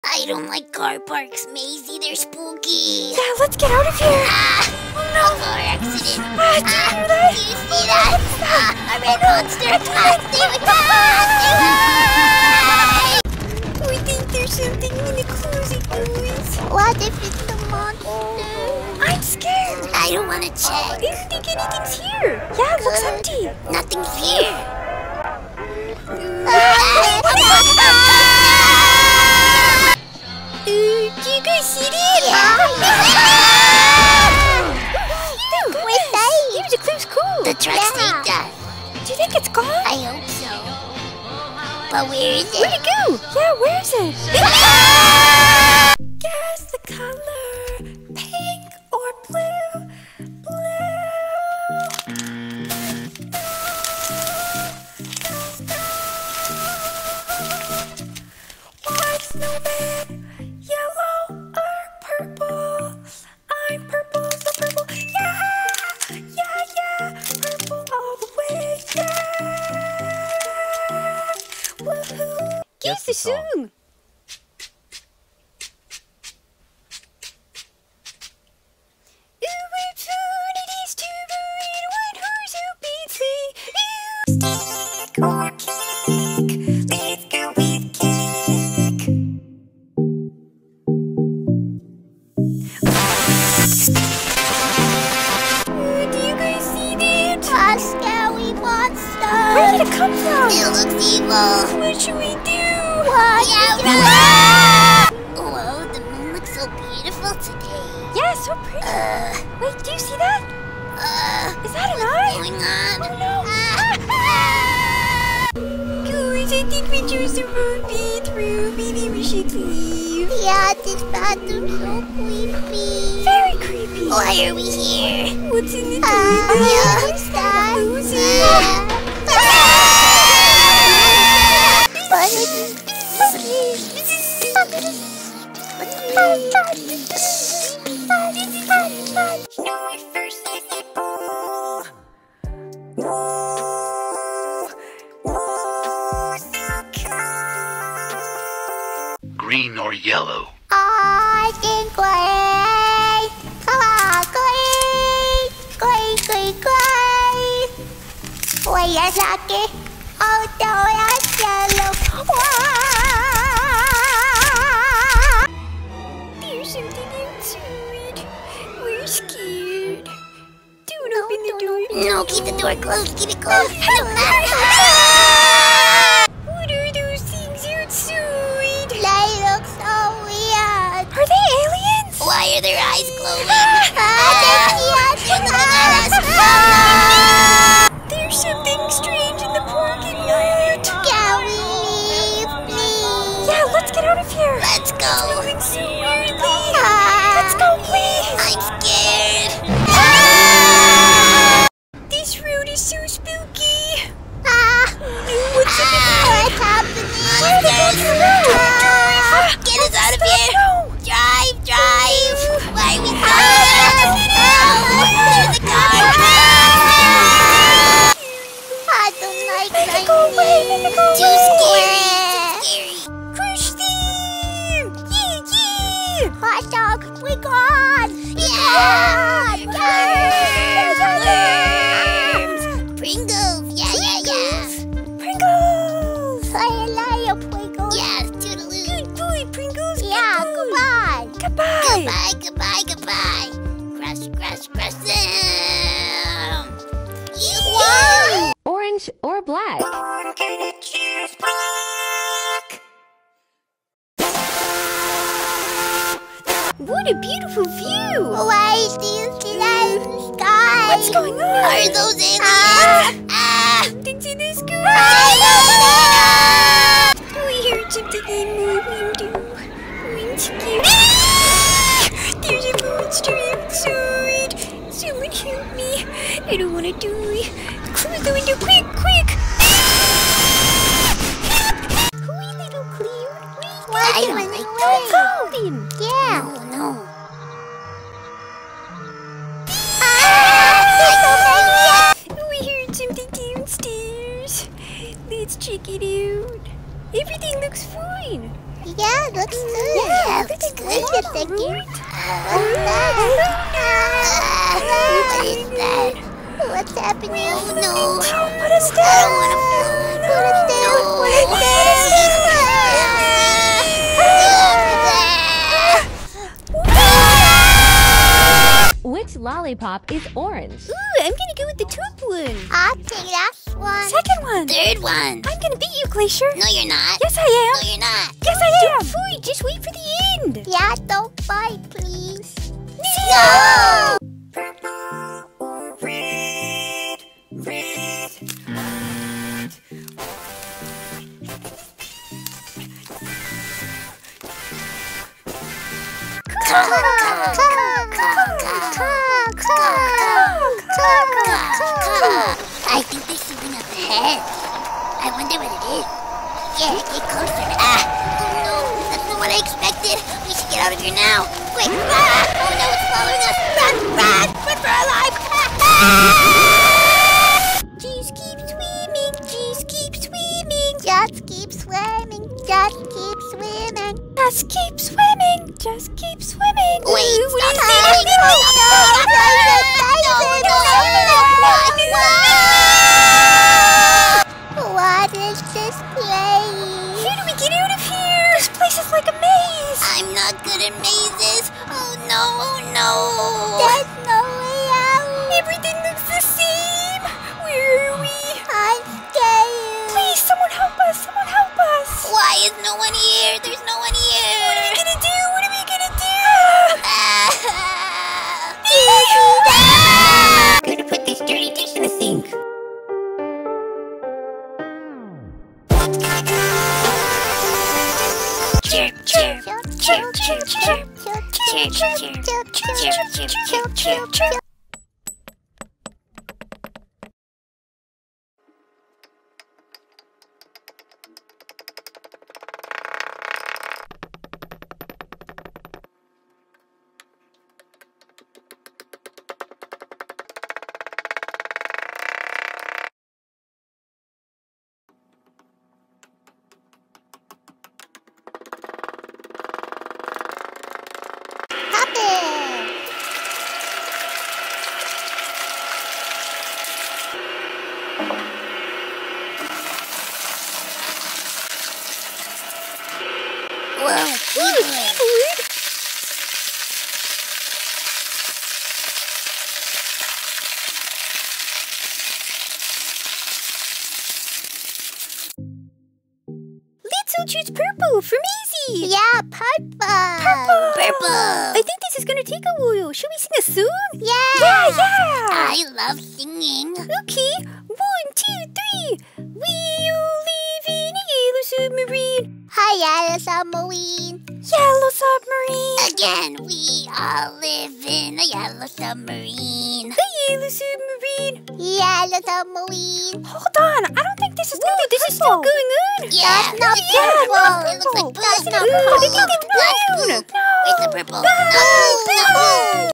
Piranhaa Plants getting FRIGHTENED for 9 minutes straight
Did you get frightened or naw? hehe 😉😂 the Plant Puppets react to Oddly Satisfying Tiktoks, funny videos, scary videos or just plain strange reacts videos! Featuring Glacier, Ever, Maisy and Piranhaa! 🔴🔵🟢🟣